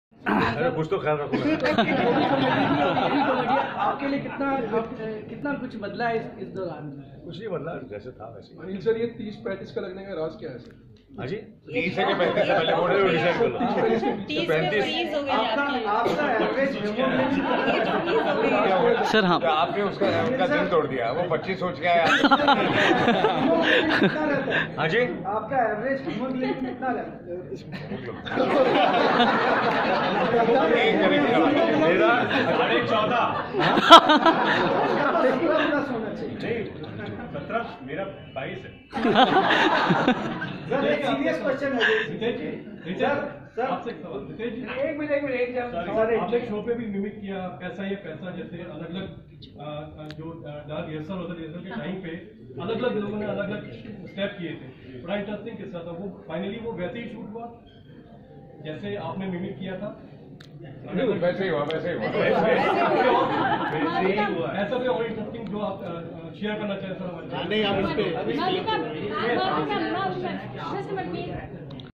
Total Dhamal star cast interview
Bol Delhi Bol ।
Star Cast of the upcoming film "Total Dhamaal" with actors Ajay Devgn, Madhuri Dixit, Anil Kapoor,Riteish Deshmukh and Director Indra Kumar in Le Meridien, New Delhi